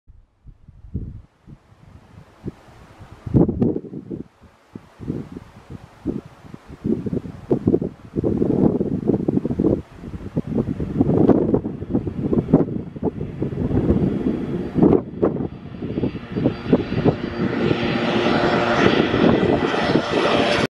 Plane taking off from Barra Beach airport
Barra Airport must be the only airport where the comings and goings of planes are dictated by the tides: it's basically a large flat beach. You can see why jumbo jets might not quite fit.